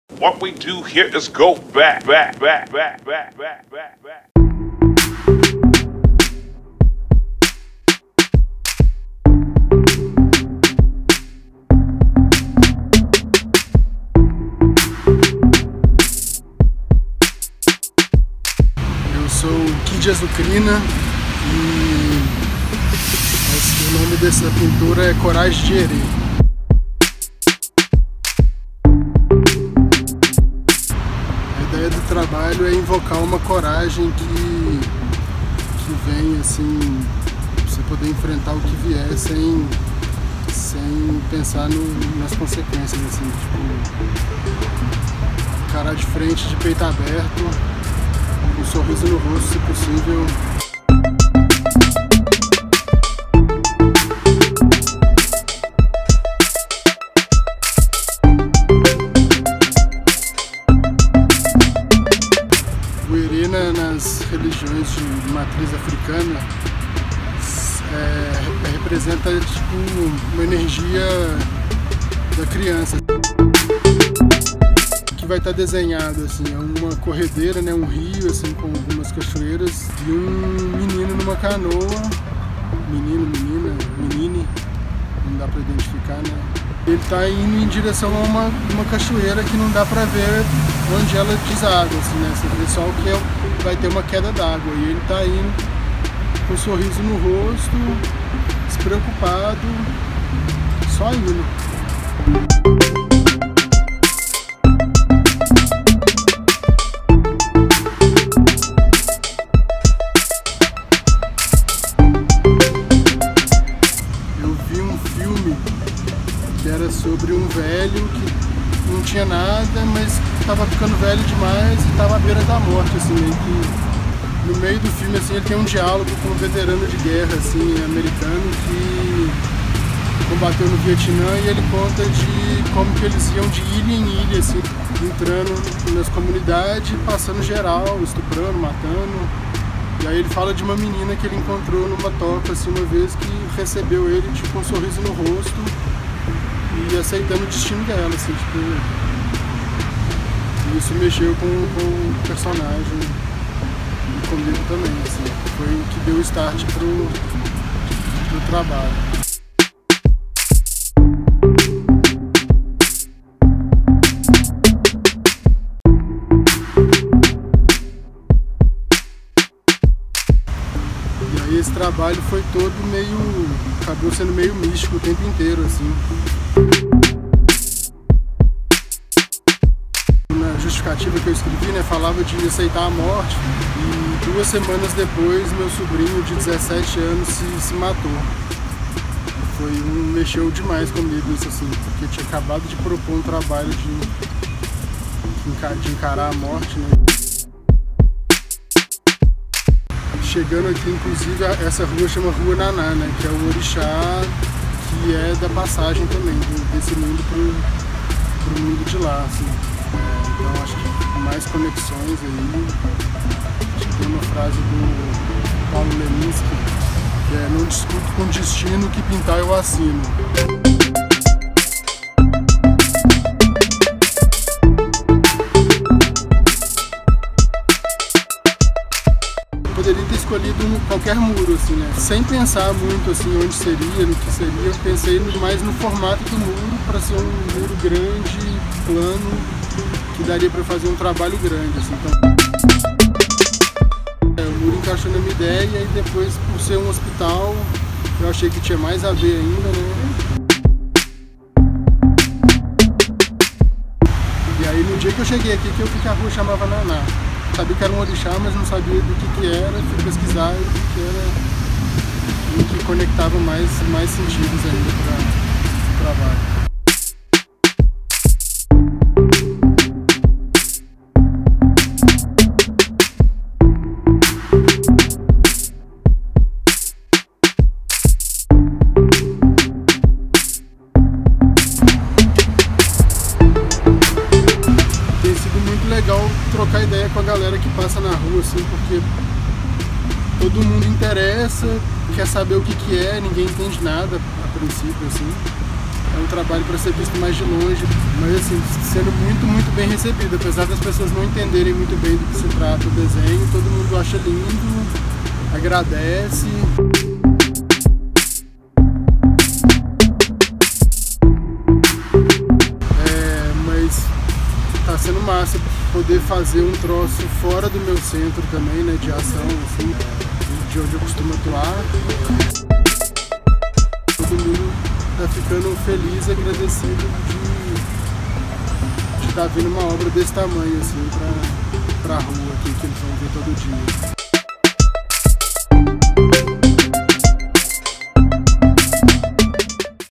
'Coragem de Erê' colore muro do Hospital Célio de Castro
Contemplado no Edital de Arte Urbana – Gentileza, da Secretaria Municipal de Cultura, o artista plástico e designer Kid Azucrina escolheu o muro do Hospital Metropolitano Dr. Célio de Castro para receber a obra ‘Coragem de Erê’.

Concluída em janeiro de 2019, o trabalho aborda a temática da invocação da coragem para o enfrentamento do destino. “Encarar de frente, de peito aberto, com um sorriso no rosto se possível”, conta o artista.

Nas religiões de matriz africana, o Erê representa o aflorar da criança que cada um guarda dentro de si.

O desenho retrata a corredeira de um rio com o Erê em cima de uma canoa que segue em direção a uma cachoeira. “Não dá para ver onde ela deságua, enxergamos apenas a queda d´água”, explica Kid.

Em quase dois meses de trabalho, o artista teve a oportunidade de conversar com muitos moradores do Barreiro que, curiosos, desejavam entender o que estava sendo pintado. “Foi muito legar trocar ideia com a galera que passa na rua. Fui muito bem recebido. Todo mundo se interessa, quer saber o que é, acha lindo e agradece”, diz.

Reportagem e roteiro: Valéria Mendes
Arte, imagens e edição: Álvaro Miranda
Realização: Assessoria de Comunicação do Hospital Metropolitano Dr. Célio de Castro